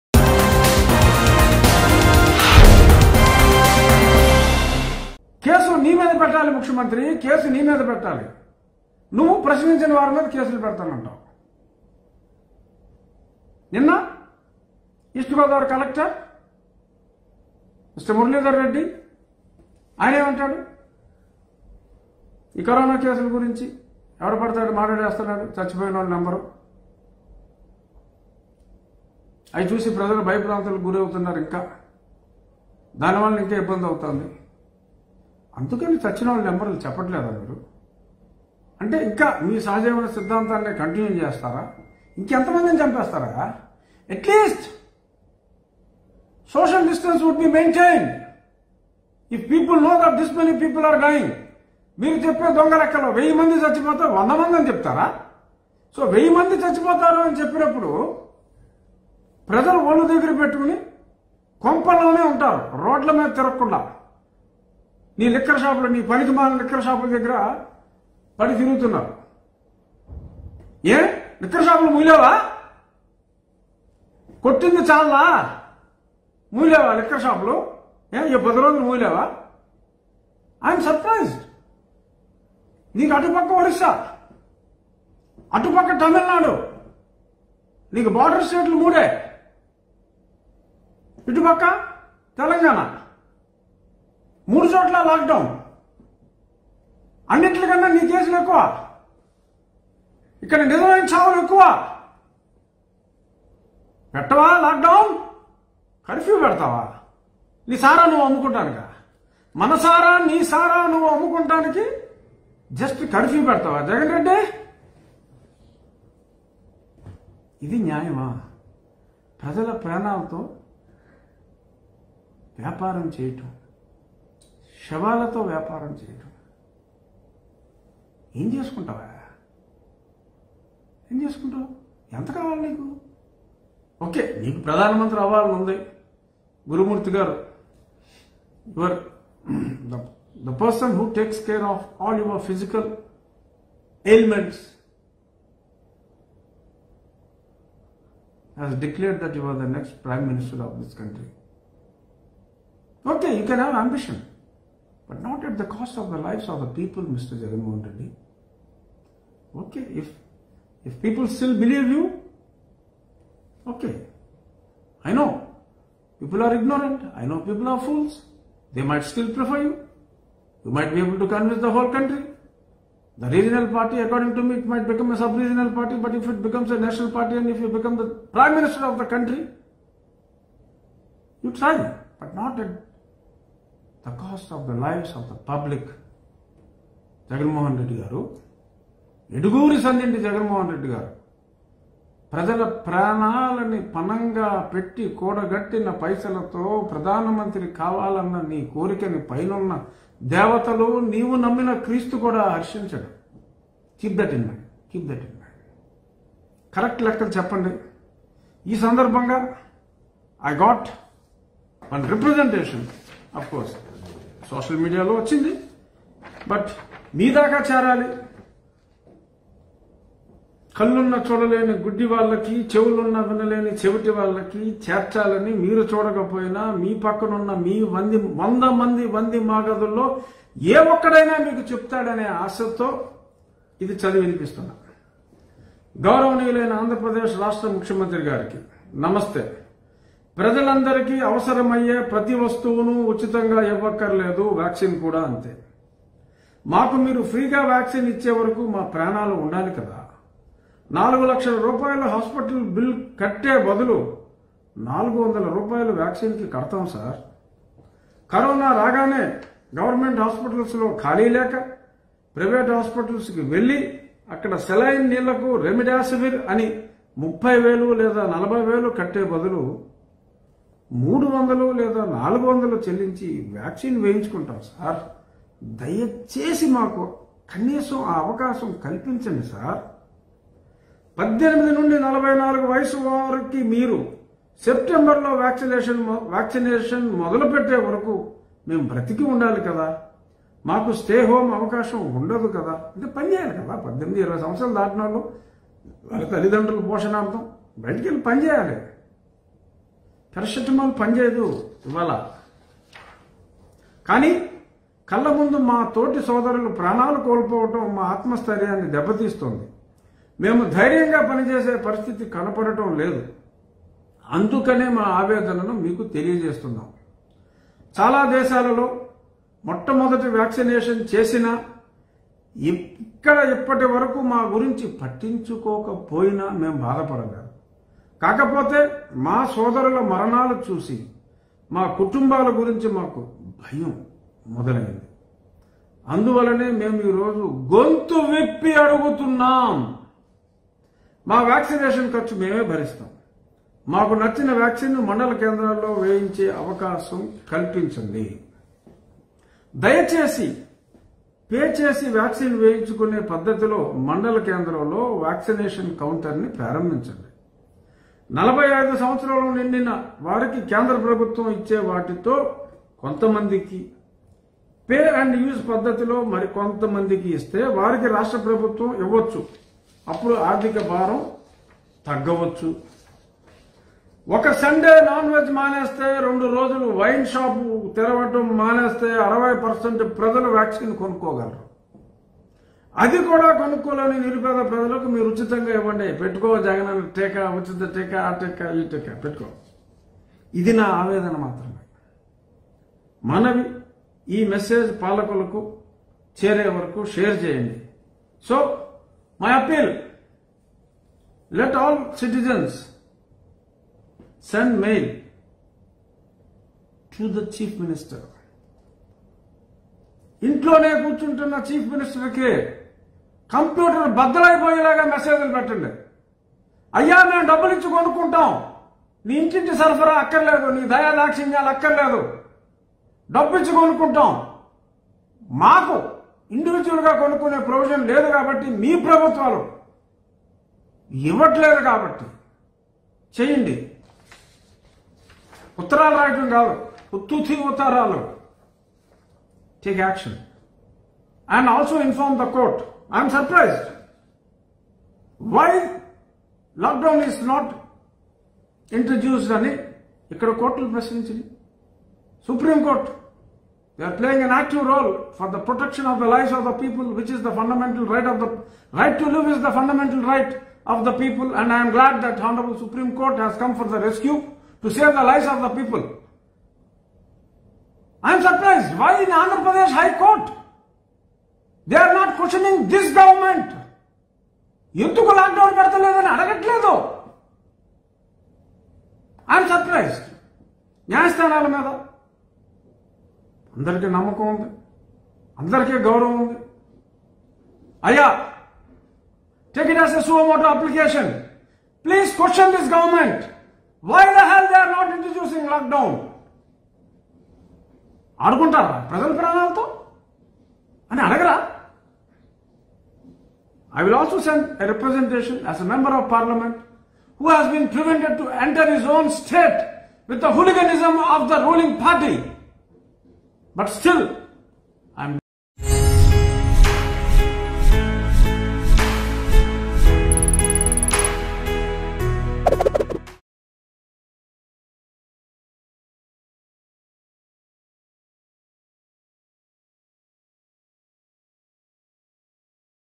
YCP MP Raghu Rama Krishna Raju Requests Govt For Full Lockdown in AP | COVID19​ | Mango News
YCP MP Raghu Rama Krishna Raju Requests Govt For Full Lockdown in AP.